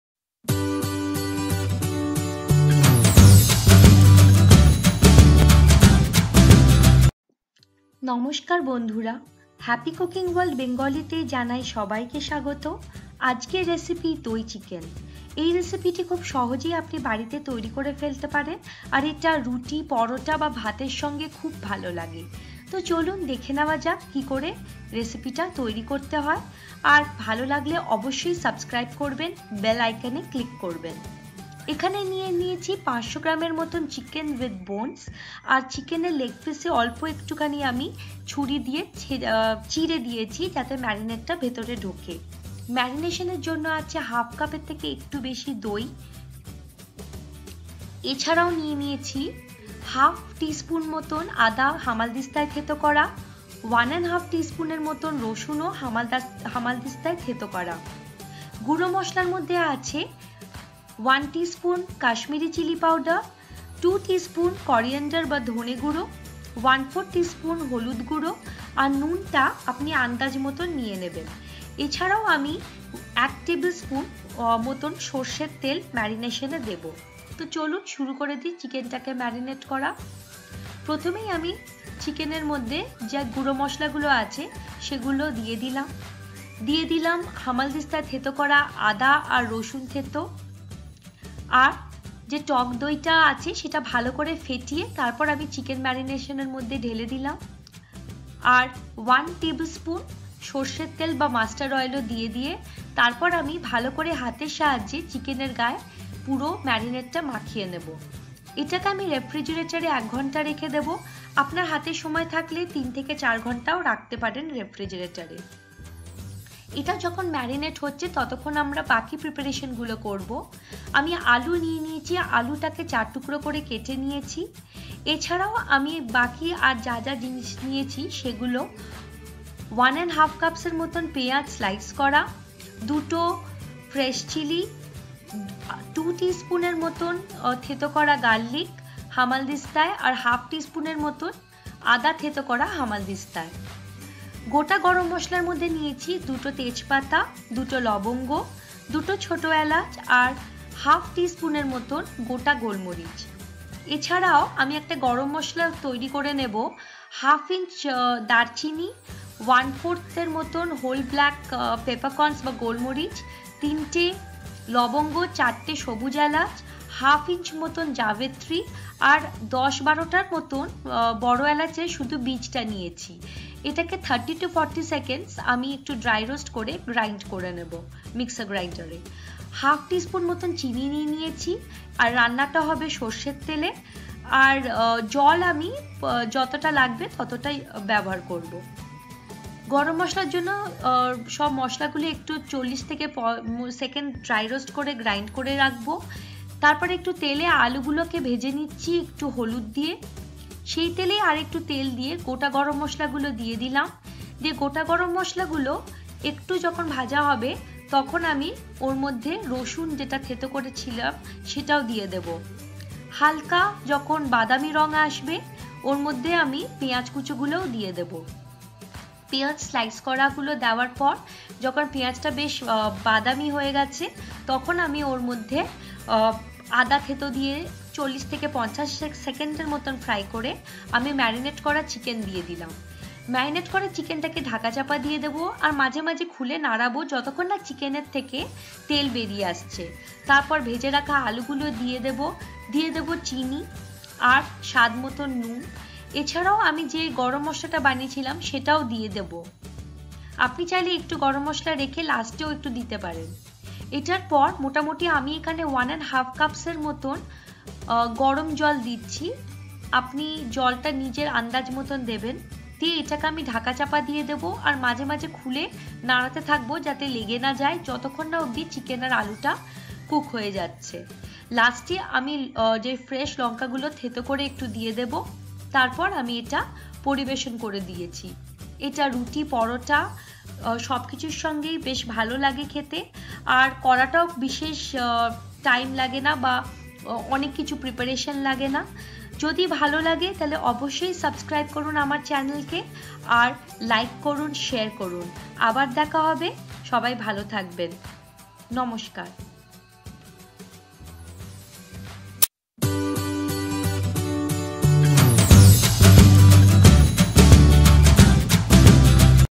Doi Chicken Recipe or Doi Chicken or Doi Murgi or Dahi Chicken in Bengali
You tube link of this recipe in English:-

Blog link for this recipe:-

1. Chicken around 1 pound (approx 500 grams)

2. Little more than 1/2 cup of Curd/Yoghurt/Doi

3. 1 and 1/2 teaspoon of Garlic crushed in a mortar and pestle/hamaldista

4. 1/2 teaspoon of Ginger crushed in a mortar and pestle/hamaldista

5. Powdered Spices ( 1 teaspoon Paprika/Kashmiri Chili Powder  + 2 teaspoon coriander/dhoney powder + 1/4 teaspoon turmeric powder + Salt as per your taste )

6. Preferably Mustard oil ( or any other cooking oil ) 1 tabelspoon1

Ingredients for the gravy :-

1. 1 and 1/2 cups of sliced onion

2. 2 fresh chili

3. 2 teaspoon of crushed garlic

4. 1/2 teaspoon of crushed ginger

5. Whole spices [2 bay leaves + 2 cloves/lobongo + 2  green cardamoms + 1/4 teaspoon- 1/2 teaspoon whole black peppercorns/golmorich  (depending on your preference)].

6. Spice mixture ( 1/2 inch cinnamon /darchini + 1/4 whole black peppercorns/golmorich + 3 cloves + 4 green cardamoms + 1/2 Mace/Javetri + Black Cardamom only the seeds around 10 seeds)dry roast on a low flame for 30 - 40 seconds and then grind it)

7. 1/2 teaspoon of sugar.

8. Mustard Oil ( 2-3 tablespoon)

9.1 medium Potato quartered ( optional )

10 . Salt as per your taste.

11. Water as needed to prepare the gravy consistency as per your preference.